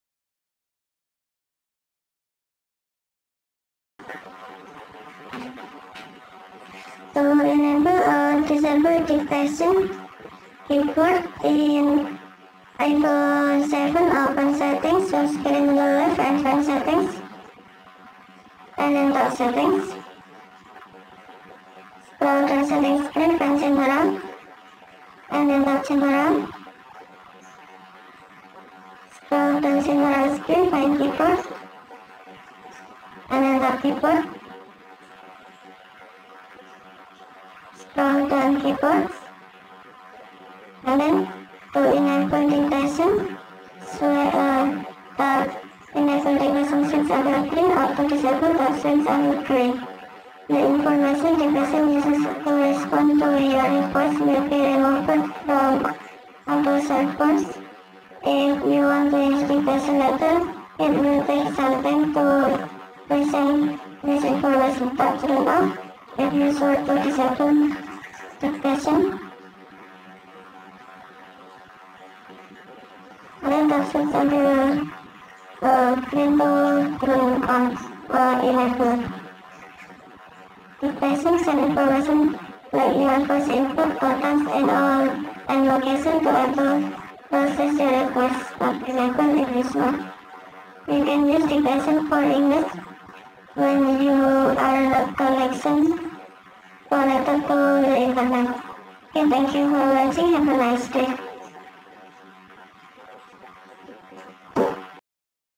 iPhone 7 : How to enable or disable dictation keyboard
On this video you can know How to enable or disable dictation keyboard in iPhone 7 with iOS 14.0.1 operating system